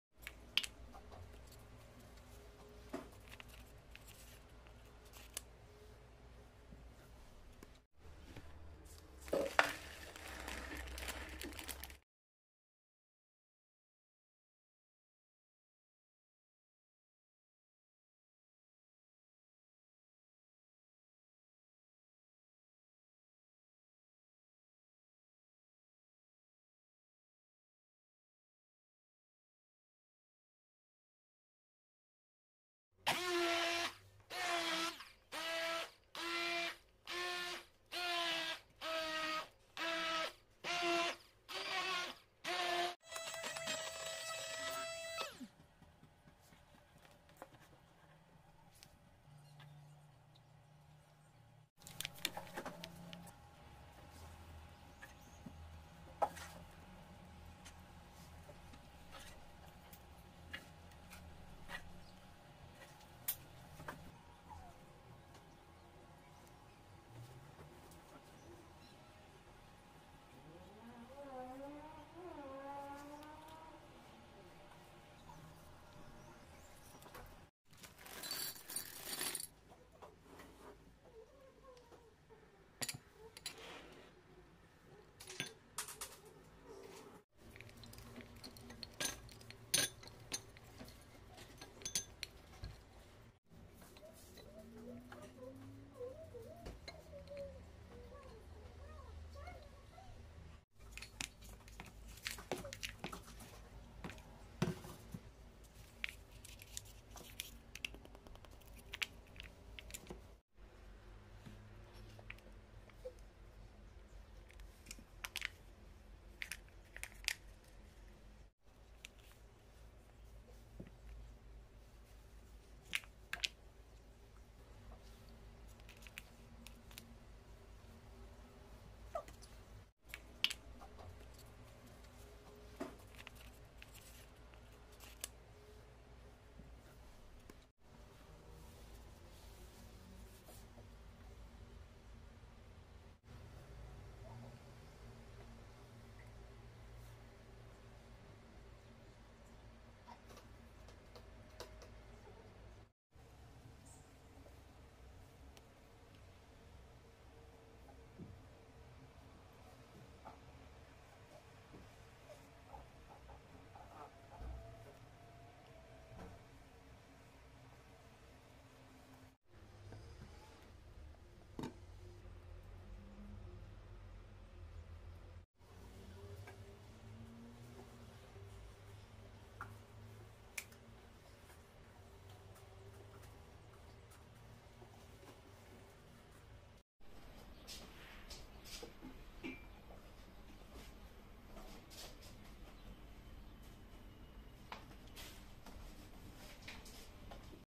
У ВАС ЕСТЬ 2 БАНАН И ТВОРОГ? Сделайте этот восхитительный десерт/ВКУСНЫЙ ЧЕМ МОРОЖЕНОЕ
Беру ТВОРОГ И БАНАН. ВЕК ЖИВИ,ВЕК УЧИСЬ. МАЛО КТО ЗНАЕТ ЭТОТ СЕКРЕТНЫЙ РЕЦЕПТ.

Шоколад 100 гр + 10 гр подсолнечное масло